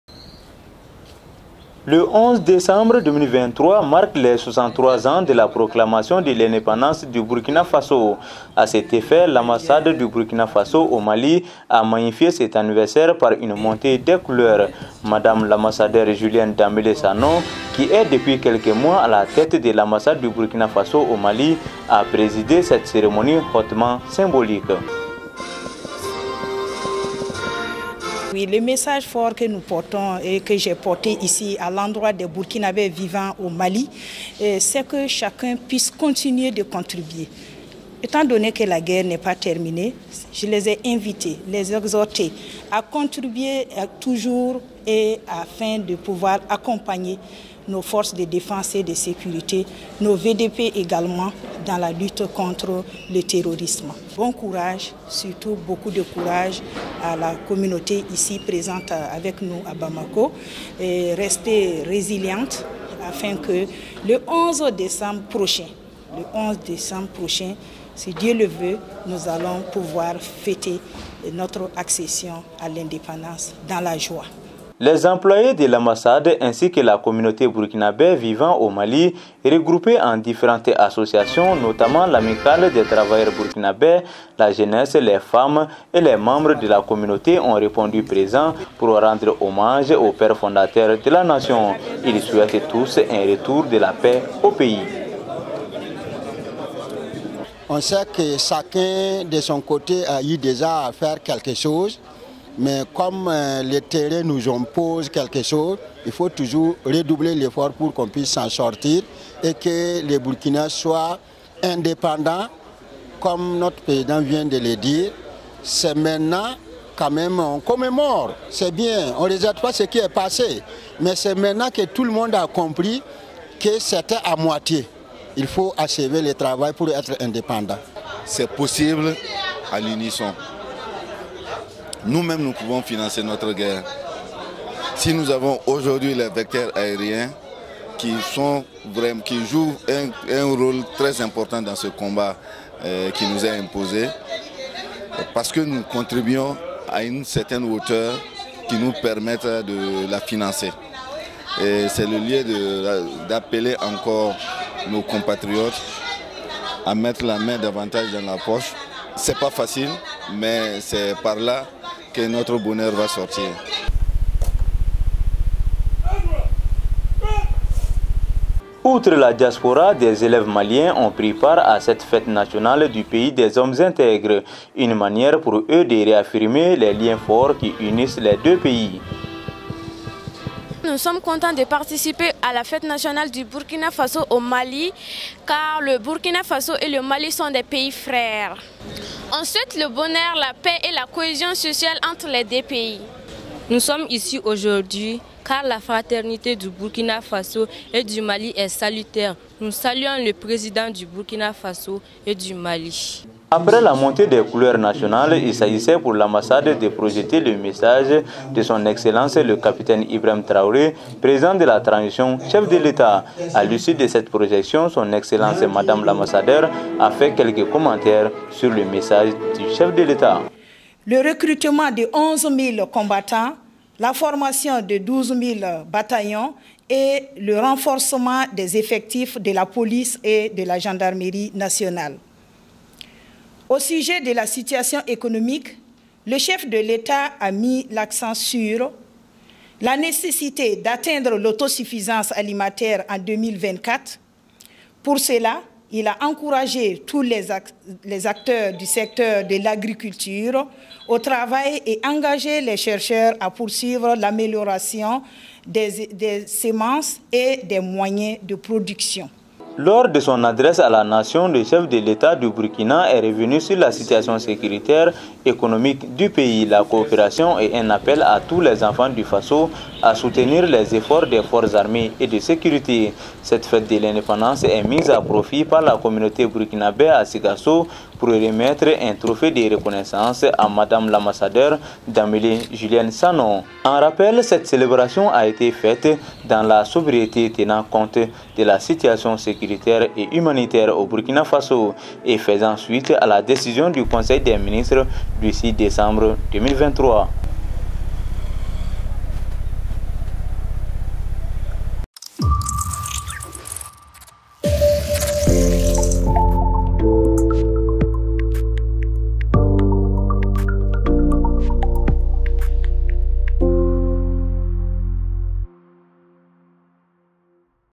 63 ans d'indépendance du Burkina Faso
11 décembre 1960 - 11 décembre 2023, cela fait exactement 63 ans que la Haute-Volta actuel Burkina Faso accédait à la souveraineté nationale et internationale.

Cette fête nationale est célébrée cette année dans la grande sobriété du fait de la situation actuelle du pays.
À l'ambassade du Burkina Faso au Mali, elle a été magnifiée par la montée des couleurs et la projection du discours à la nation du chef de l'État.
En plus du personnel de l'ambassade, ils étaient nombreux, les Burkinabè à témoigner de leurs présences à cette cérémonie.